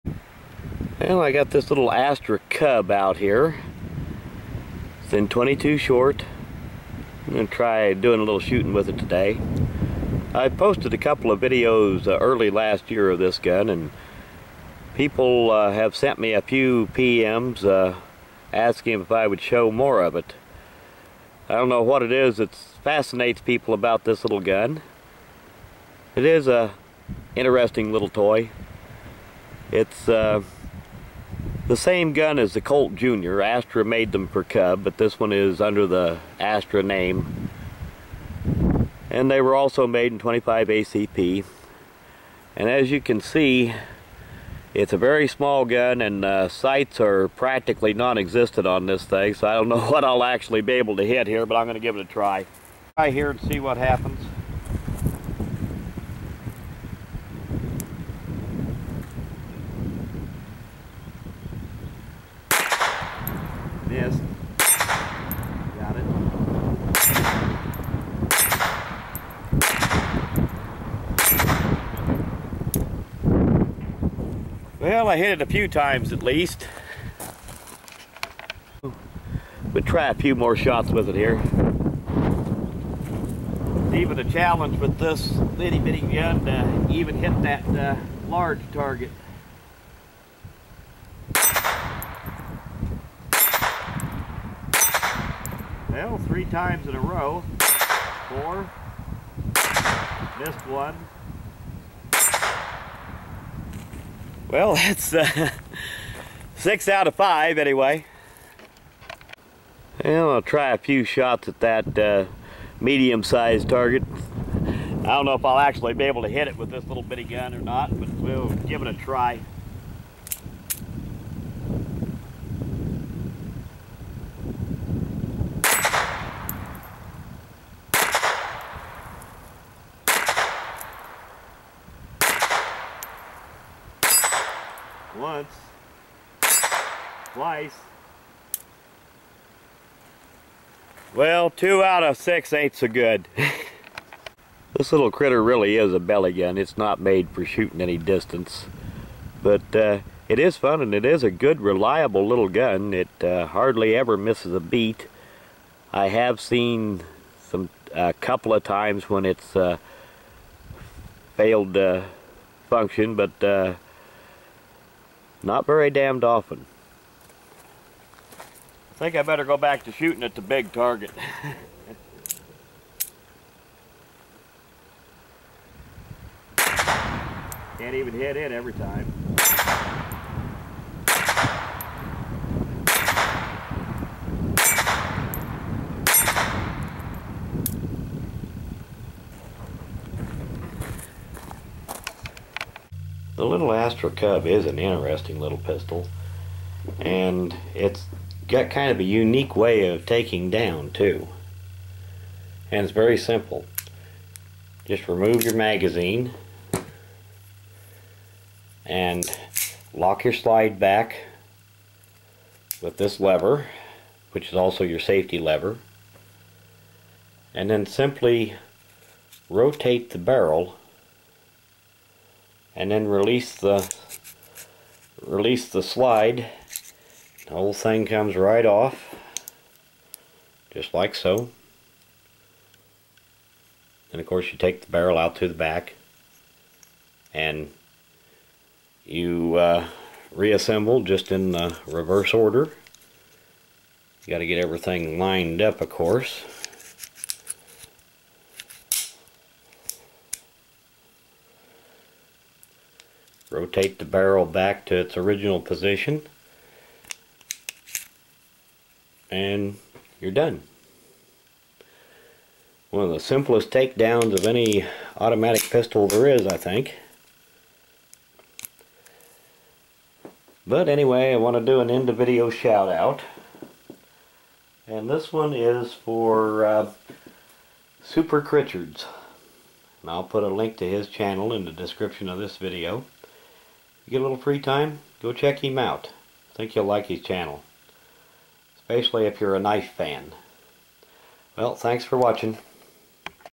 Astra Cub 22 Short Pocket Pistol
The Astra Cub was made in both 22 short and 25acp. A small pocket gun imported in the 1960. Same gun as the Colt JR. The Colt Jr. was made for Colt by Astra.
Importation of this pistol was ended in 1968 because it no longer qualified for importation due to the 1968 gun control act.
Unlike many of these small pocket guns, the Astra Cub/Colt Jr. pistol is good quality and reliable. But it is very underpowered by todays standards.

End of video shout out is for SuperCrichards.